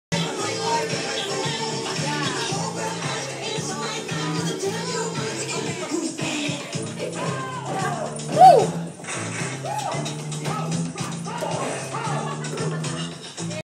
halloween firas Vs Jesse dance off
Halloween 2009
firas and jesse having a dance off
well jesse knew he didn't have much a chance, was plotting to club firas after..